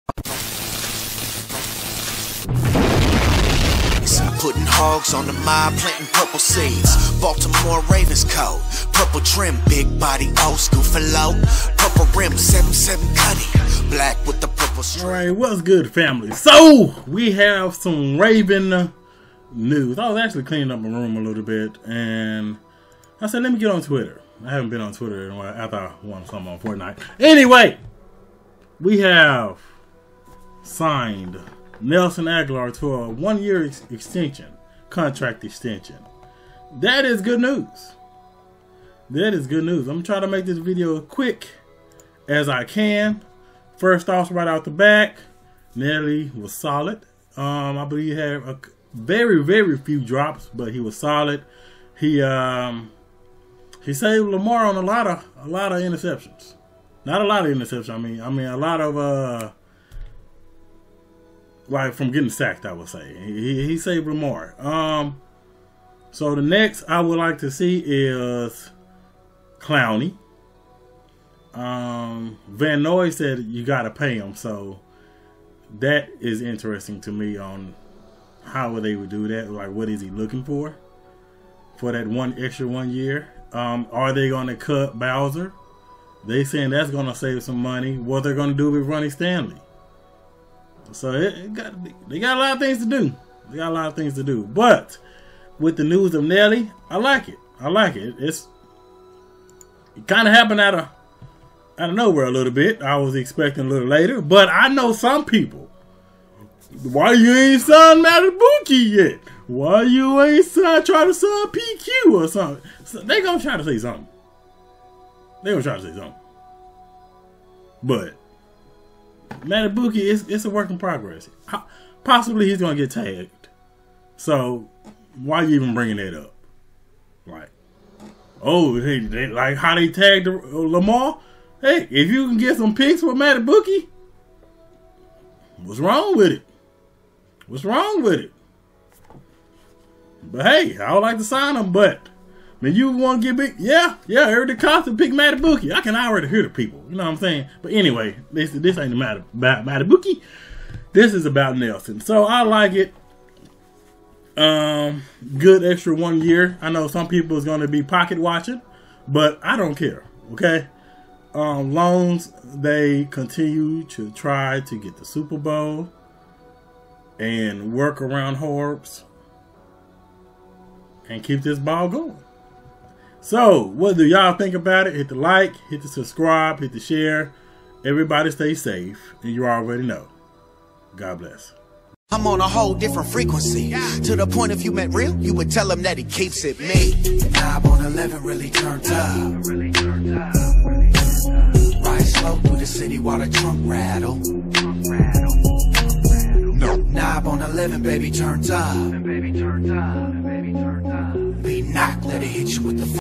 Ravens making early moves this just the start Nelson Agholor to a one year contract extension!
📫 Want to send something to me: RavenCee